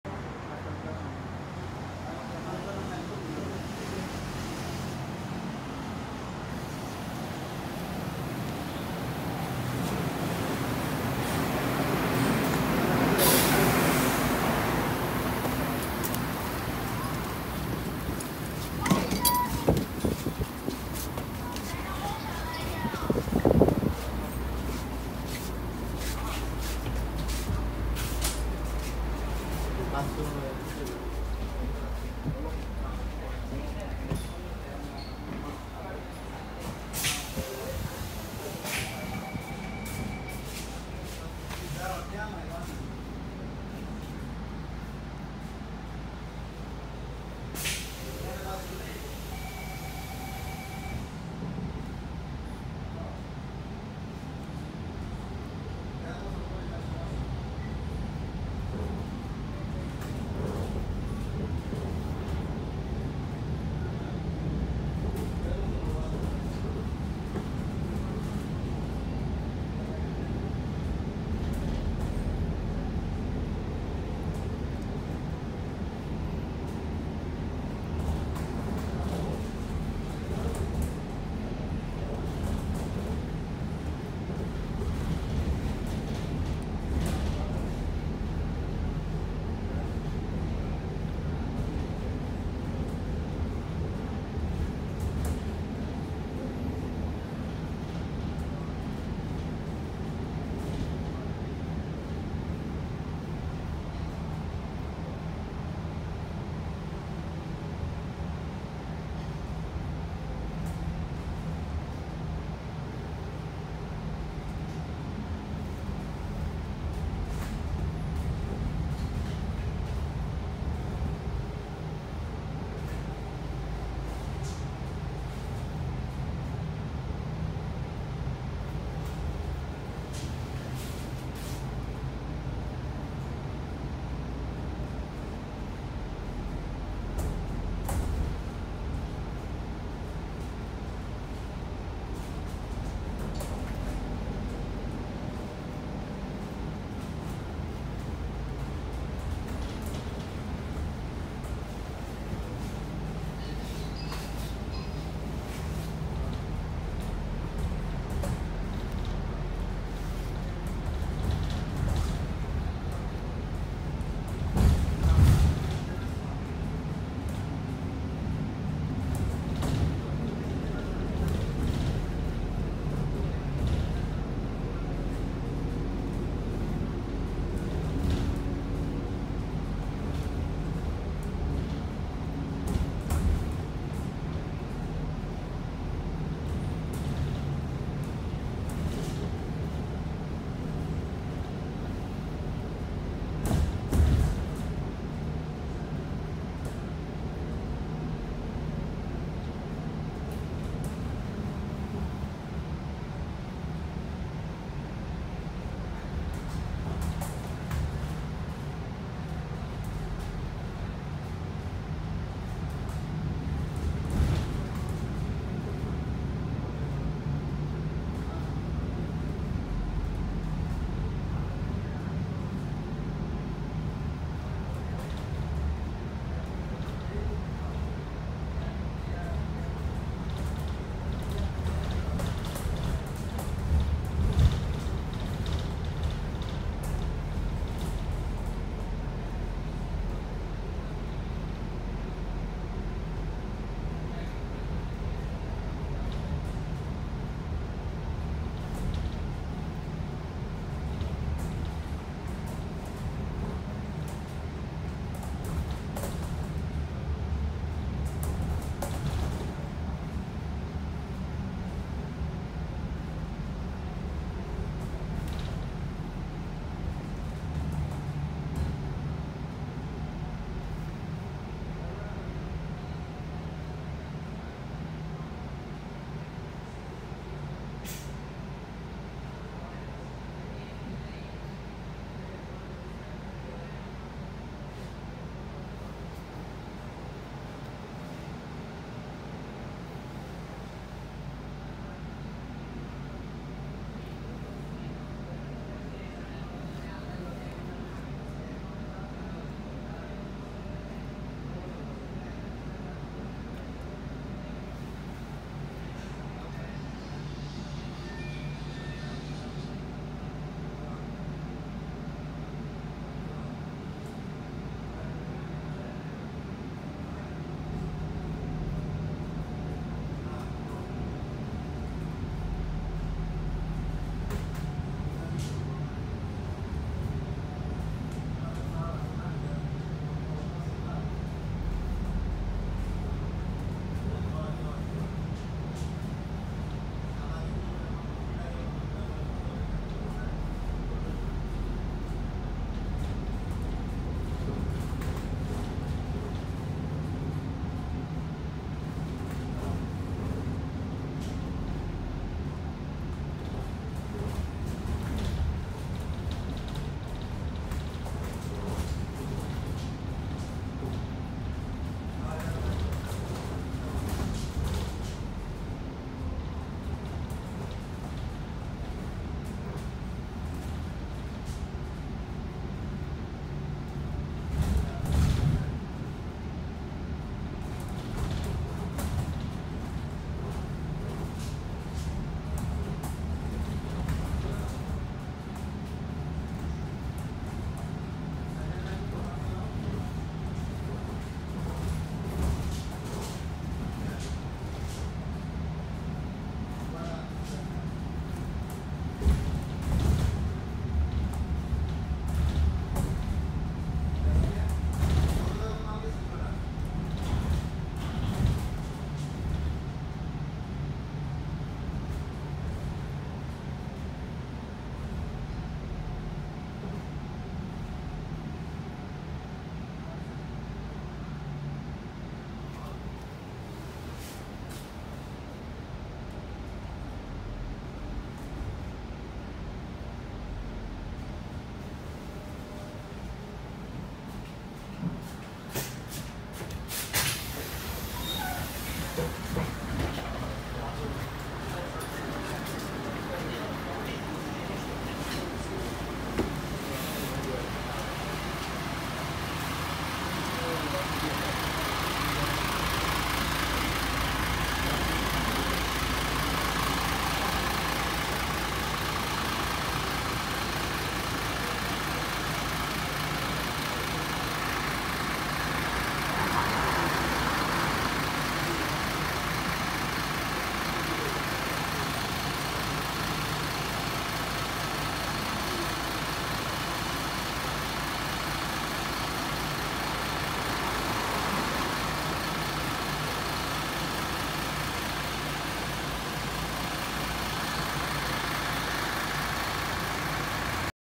SMRT MAN ND323F A95 Euro 5 - SG5814Z on EWL Free Shuttle Bus (Apr 2018)
ThrowbackThursday to one of the free shuttle bus routes were plied between Gul Circle and Joo Koon Stations before the new CBTC on the East-West Line has implemented.

Here is the eastbound routing of the EWL Free Shuttle towards Joo Koon Station, in April 2018.